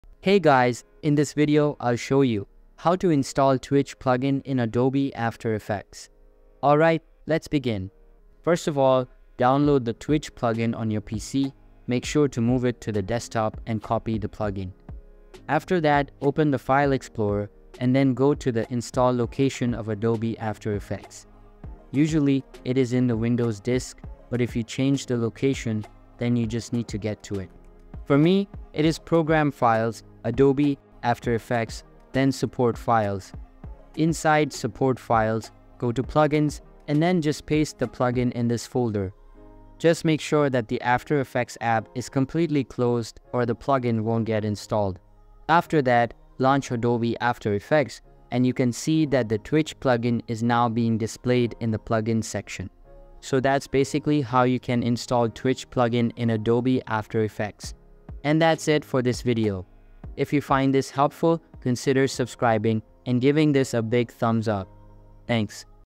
How to Install Twitch plugin in Adobe After Effects CC
Learn how to enhance your video editing skills with the Twitch plugin for Adobe After Effects CC! This step-by-step tutorial will guide you through the installation process, allowing you to add dynamic effects and transitions to your projects effortlessly. Watch now to discover how to install the Twitch plugin and unlock a new world of creative possibilities in After Effects!